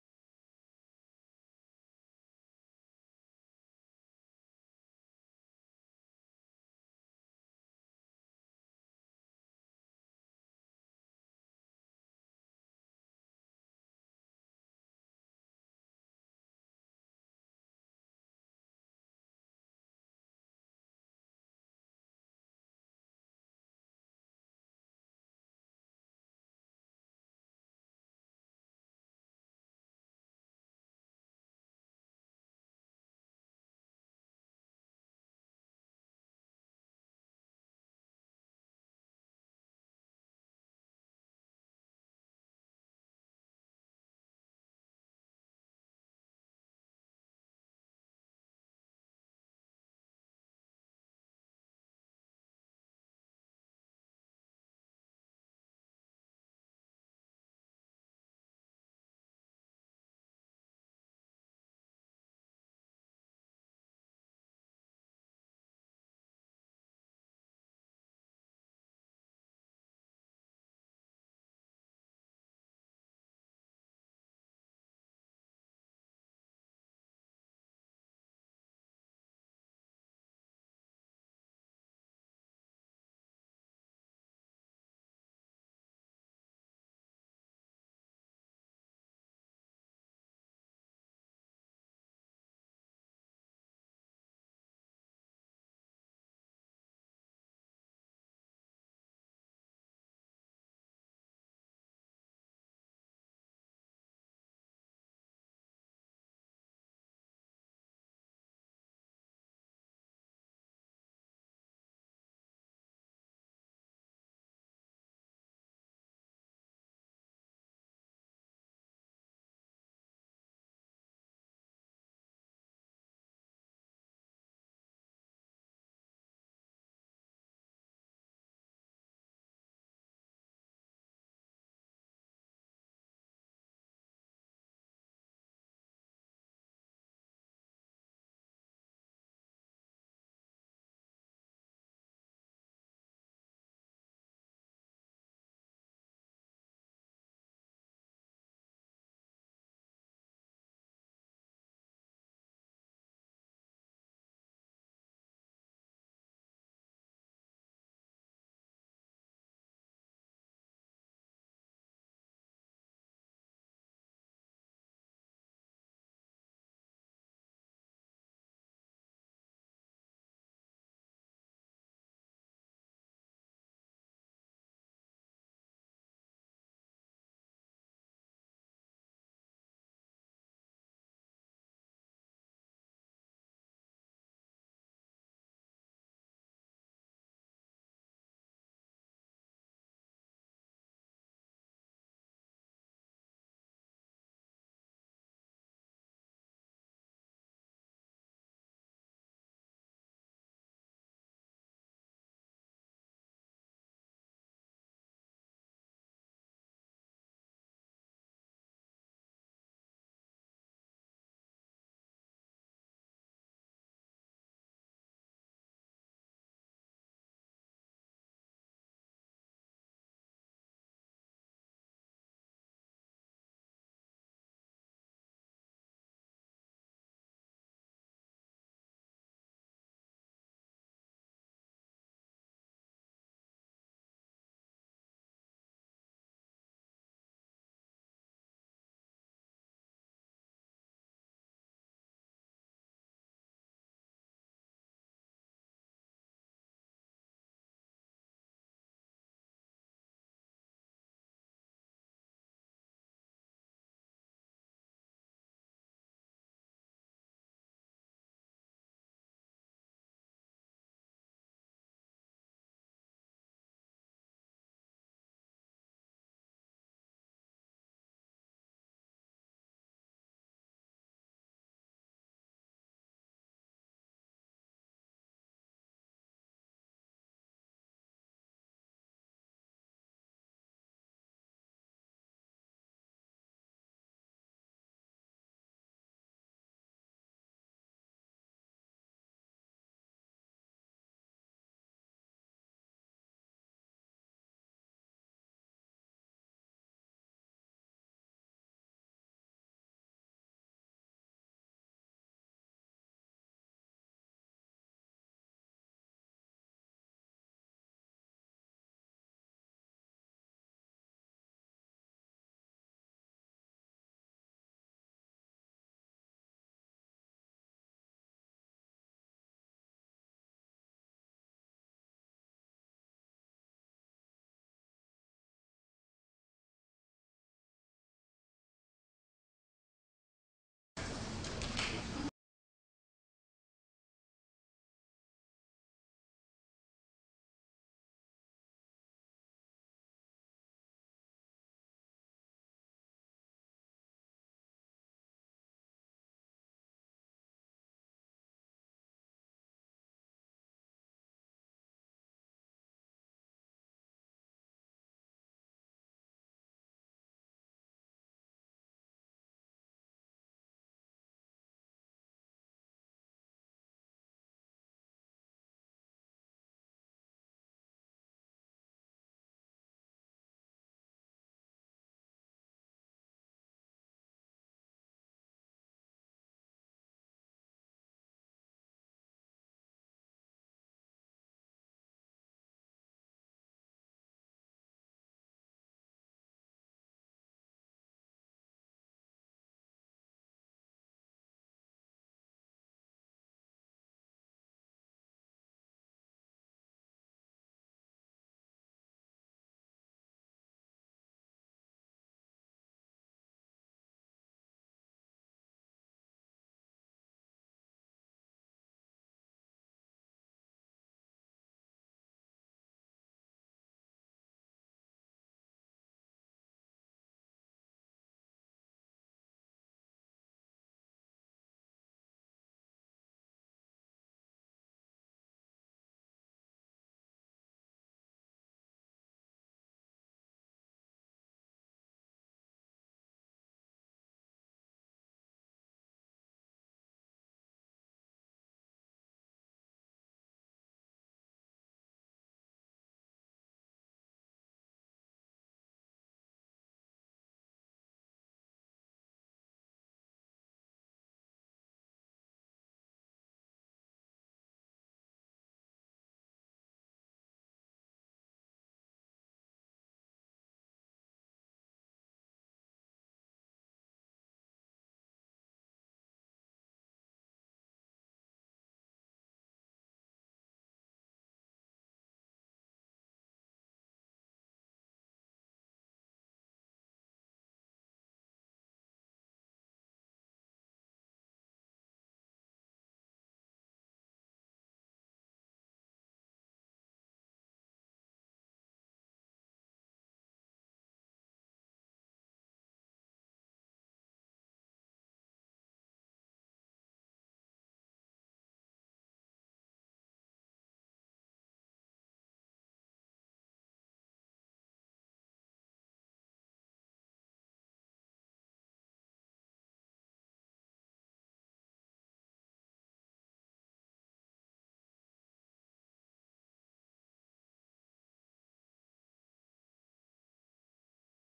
ECGOP.mov
This shows the common sense solutions that the Freedom Institute of Erie County has put forward to reform the Erie County GOP